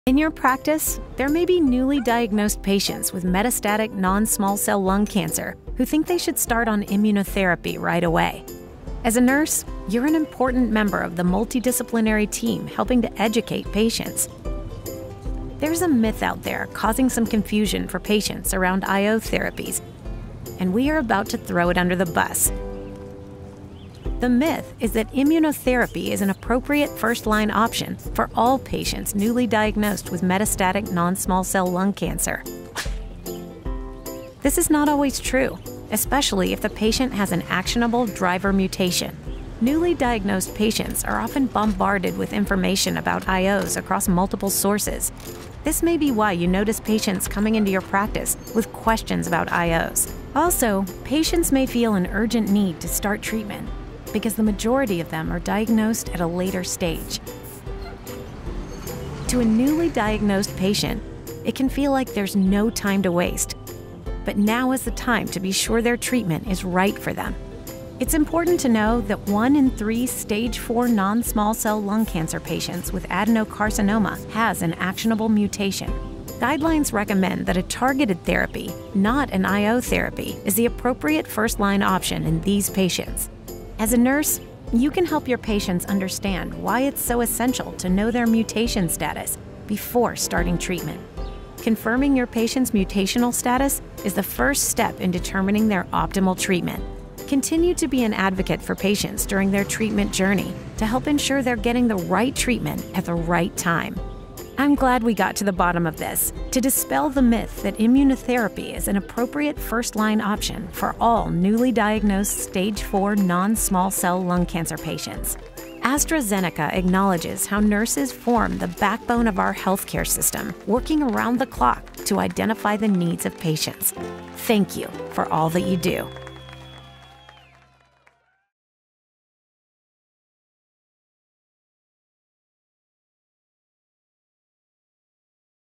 IO/TKI: Myth Busted
Watch this short, animated video that debunks the myth that immunotherapy is an appropriate first-line option for all newly diagnosed patients with metastatic non-small cell lung cancer (NSCLC).